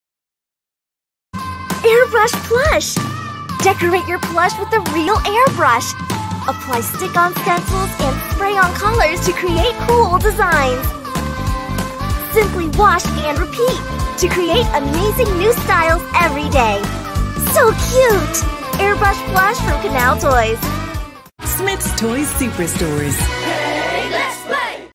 Airbrush Plush Wild Cat - Smyths Toys
Give your Wild Cat a style makeover! Customise your plush in endless ways using the cool battery powered airbrush, 4 brightly coloured chalk-based markers & more than 100 stencil patterns!